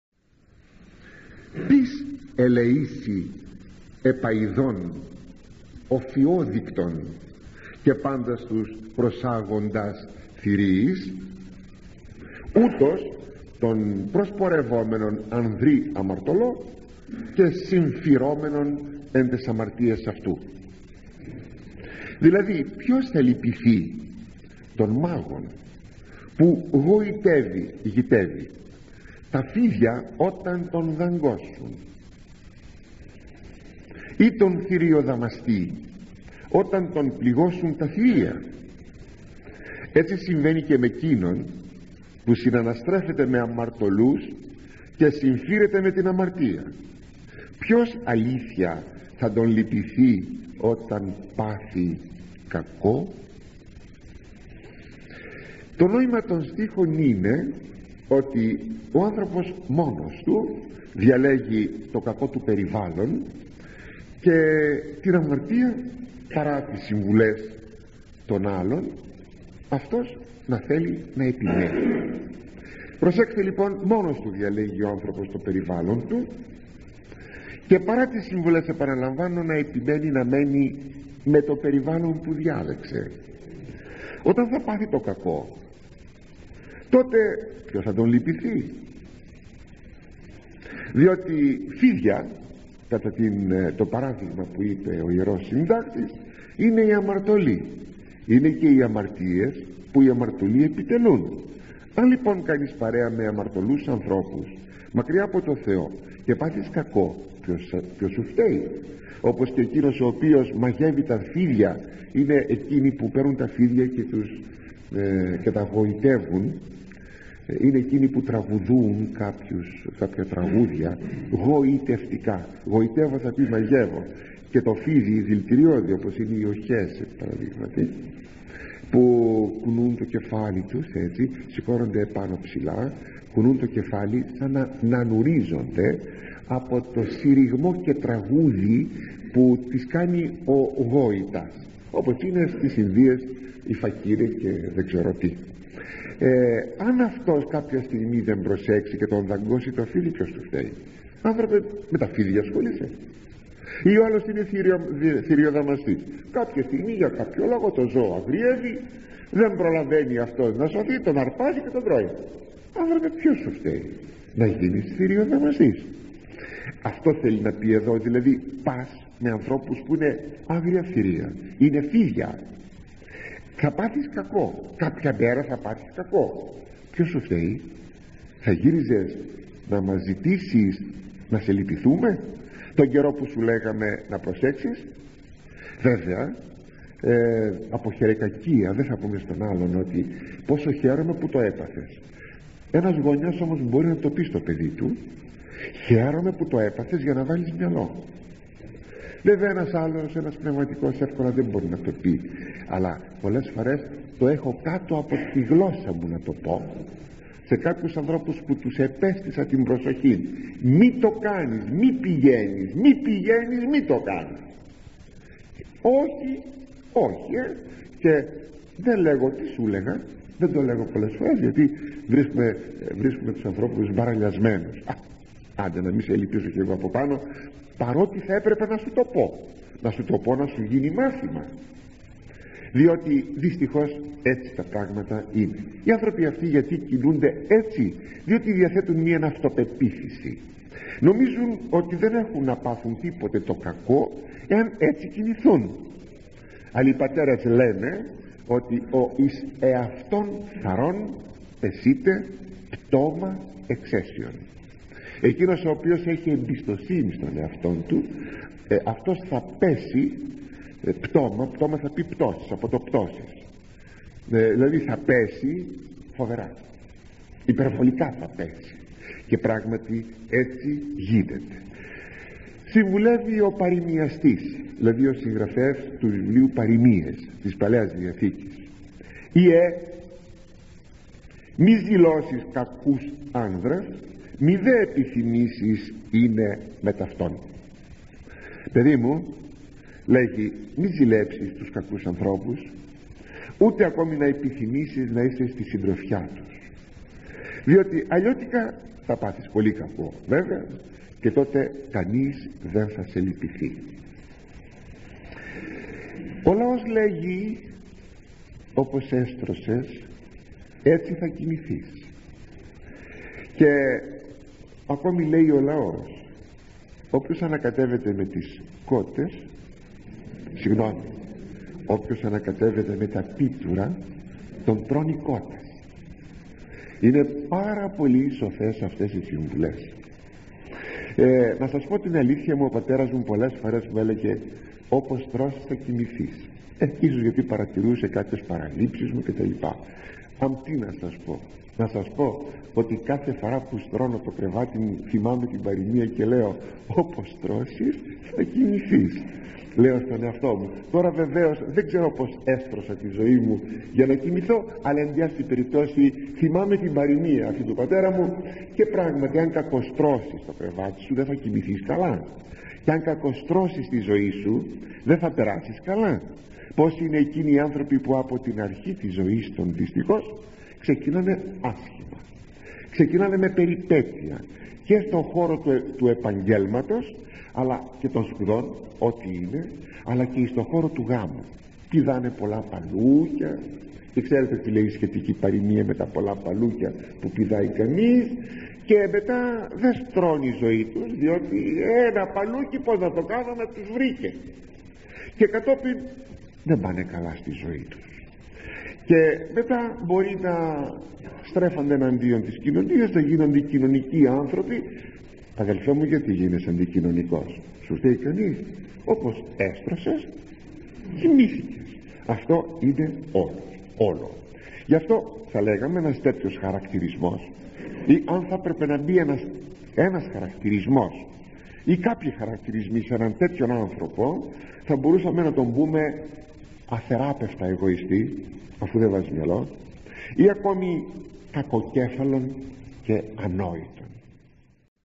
Ὁ καθένας διαλέγει μόνος του τό περιβάλλον του.
► Κατηγορία :
" Σοφία Σειράχ "

01/08/89

Κεφ 12, 13 - 18

Διάφορες πλευρές συμπεριφορᾶς ἑνός ἐν εὐρείᾳ ἐννοίᾳ ἐχθροῦ.
 Ὀφείλομε νά διαθέτωμε πολλή διάκρισι.
 Ἄν παρασυρθοῦμε, ἀποδεικνυόμεθα ἀλάτι ἀνάλατο.
 Ἄν μείνωμε ὄρθιοι στίς προσβολές ἔχομε τόν ἔπαινο τοῦ Κυρίου.

►Όλες οι ομιλίες της Κατηγορίας :
" Σοφία Σειράχ " εδώ ⬇️